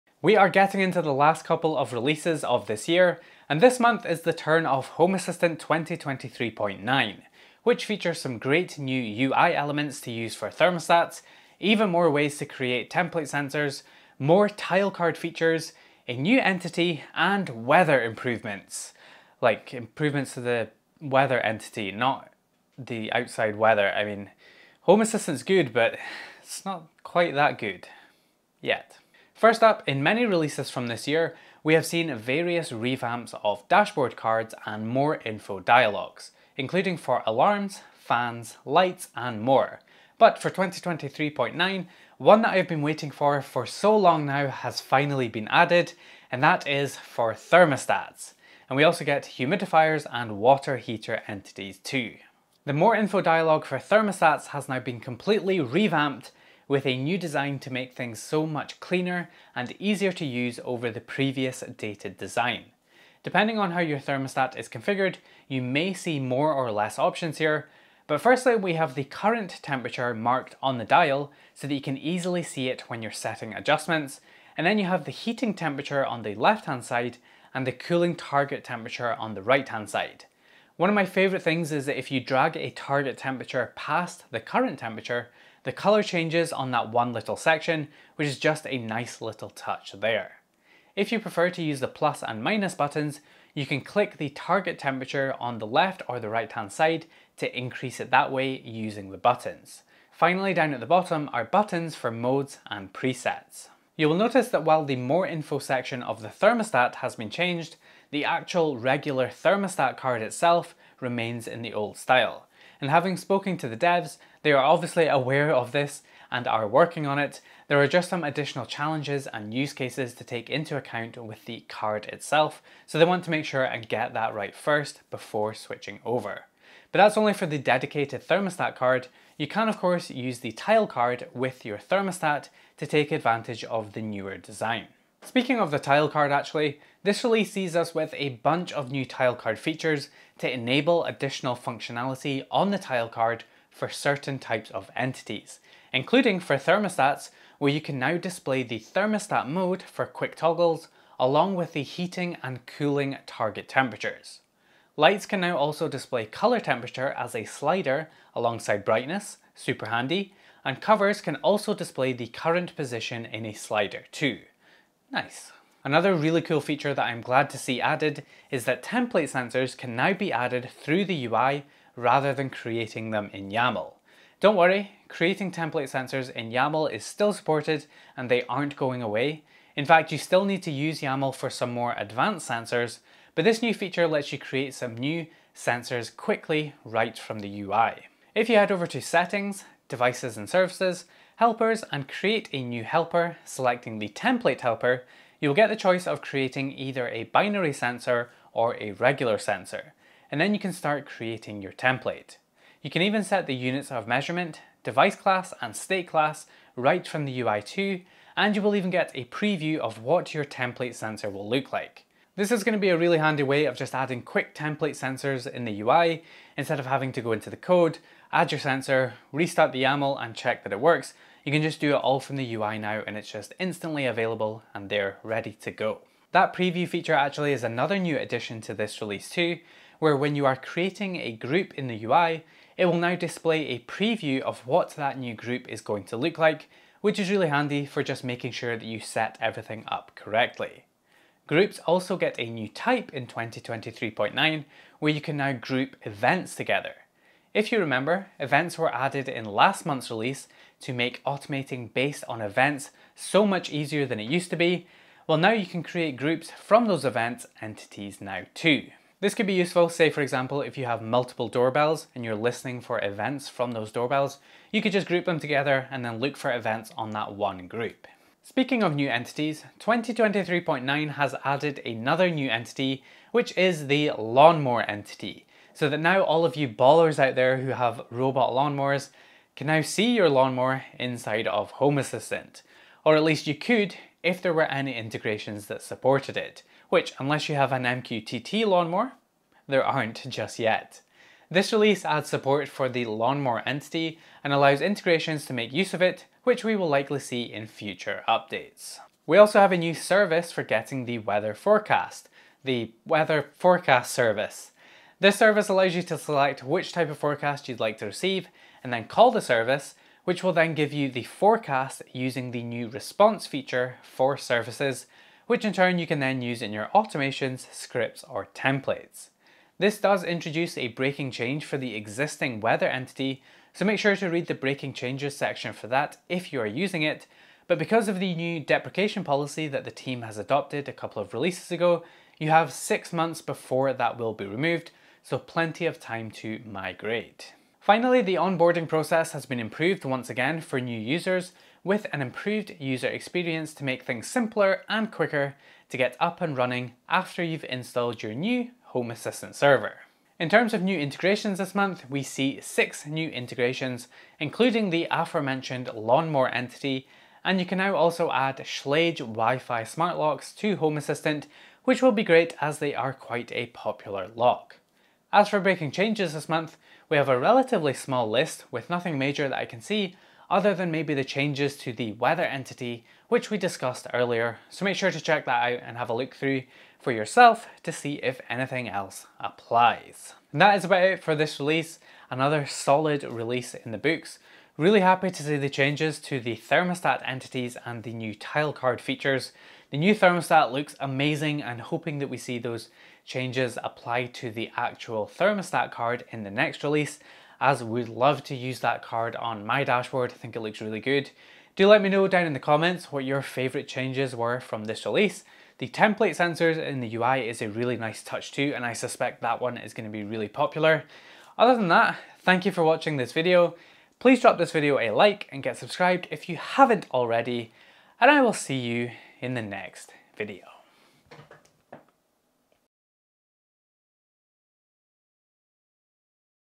Everything New In Home Assistant 2023.9!
Home Assistant 2023.9 has arrived and this month brings in new dialogues for thermostats, water heaters and humidifiers, more tile card features for lights, covers and thermostats, new entity types, weather improvements, an improved onboarding process and so much more!

EVERYWHERE YOU CAN FIND ME:
Everything Smart Home Community Discord: